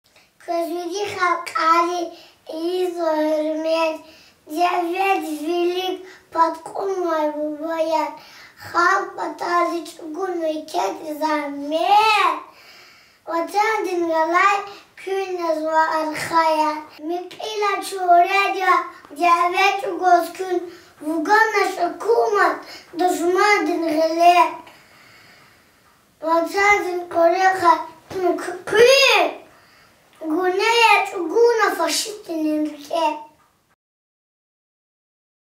Гаджибалаев Амир Ш. Стихотворение ко дню 9-Мая.
9-лагьай майдиз талукьарнавай флешмобдин иштиракчи. Аленушка бахчадин юкьван группадин Гаджибалаев Амира Сажидин Саиджамаловичан шиир к1елнва.